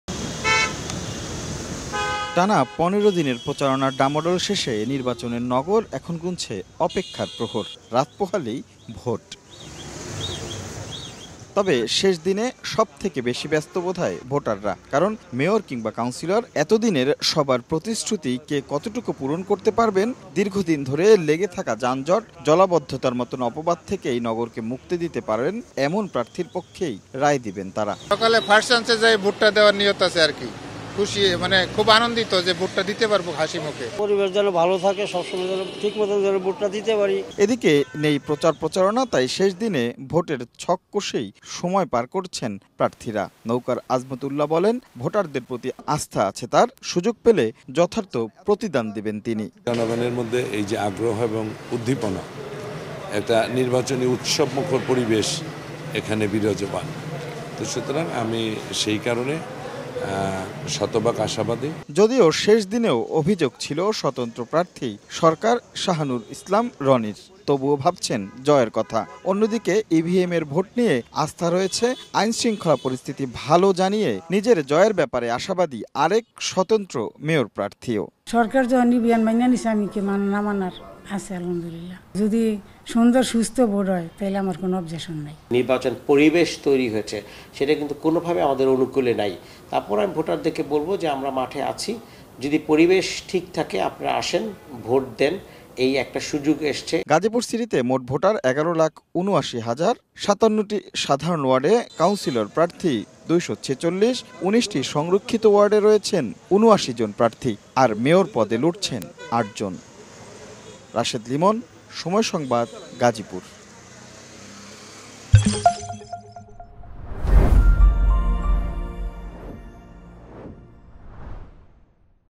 রাত পোহালেই ভোট, ছক কষছেন প্রার্থীরা | Gazipur City Corporation Election | Somoy TV
ভোটের আগের দিন ছক কষেই সময় পার করছেন প্রার্থীরা। ভোটারদের প্রতি আস্থা রেখে নৌকার আজমত উল্লাহ বলেন, সুযোগ পেলে যথার্থ প্রতিদান দেবেন তিনি। আর জয়ের ব্যাপারে আশাবাদী স্বতন্ত্র মেয়র প্রার্থী সরকার শাহনূর ইসলাম রনিও। এদিকে, গাজীপুরকে আধুনিক নগরী হিসেবে গড়ে তুলতে পারবেন এমন প্রার্থীকে রায় দেয়ার কথা জানান ভোটাররা। রাশেদ লিমনের রিপোর্ট।